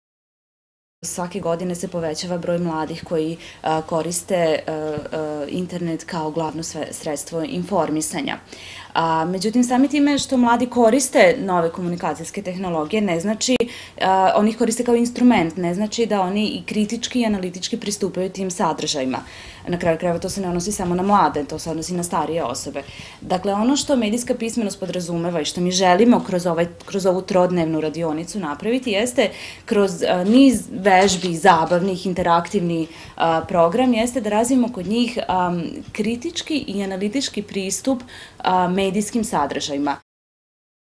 Trening o medijskoj pismenosti
Udruženje „Kult" u saradnji sa Kancelarijom za mlade, organizuje besplatan trening na temu :"Kako pametno koristiti medije i biti medijski pismen". Trening je namenjen mladima od 17-30 godina, a prijave u vidu biografije i motivacionog pisma zainteresovani mogu poslati na email: , dok više informacija mogu dobiti na broju: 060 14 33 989. Radionica će biti održana u prostorijama Saveza strukovnih udruženja, a vodiće je Vanja Ibrahimbegović Tihak iz organizacije Internjuz iz Sarajeva, koja je prošla obuku pri američkom Centru za medijsku pismenost. Cilj radionice je naučiti polaznike da kritički sagledaju medije, da nauče sagledavati priče iz sopstvene perspektive i prepispitaju poruke i stavove koji se nalaze u dominantnom medijskom sistemu. Rok za prijavu je 30. januar, a trening će se održati 7, 8 i 9. februara.